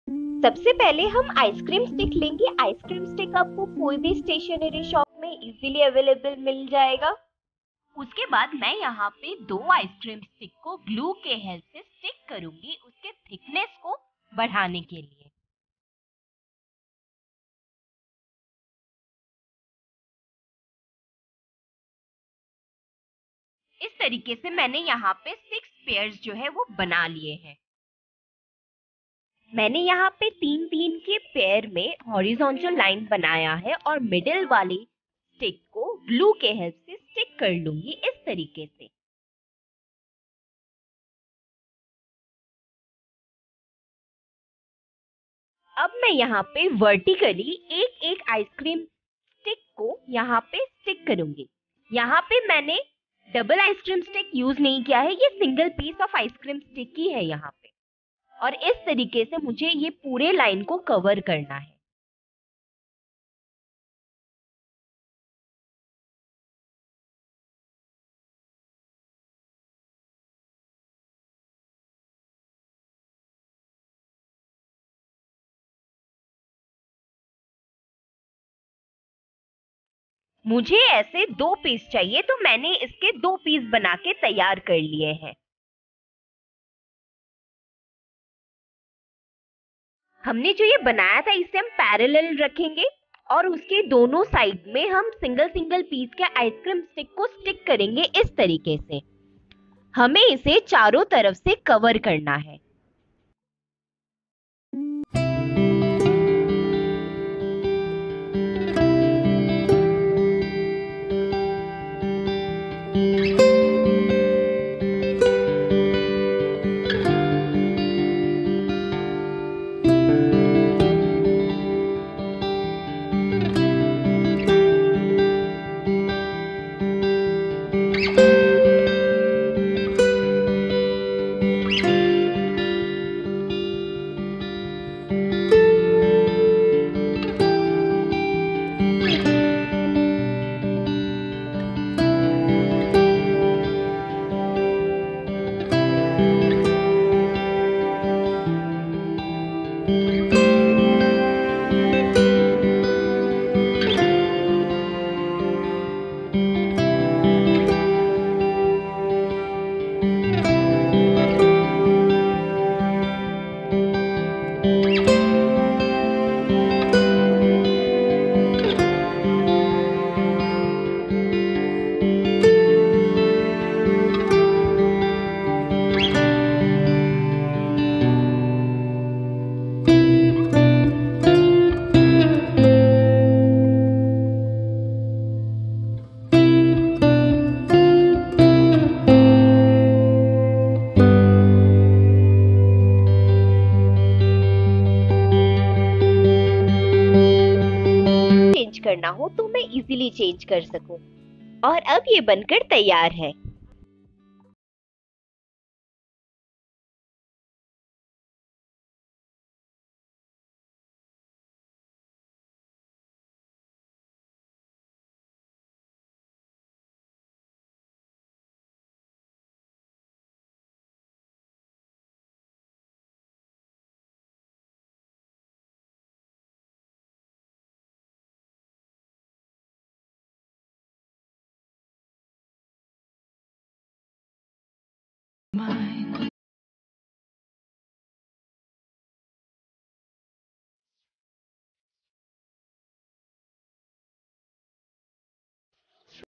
DIY | Table Lamp For decoration | Home Decor | With Ice Cream Sticks
In this video we make table lamp with the help of Ice cream sticks.
You can use this lamp as for decoration or for your room as night lamp.

For making this lamp I use
1. Ice cream sticks
2. Hot Glue Gun
3. Holder
4. Bulb
5. Wire for connection

You can make this lamp easily in your home by yourself.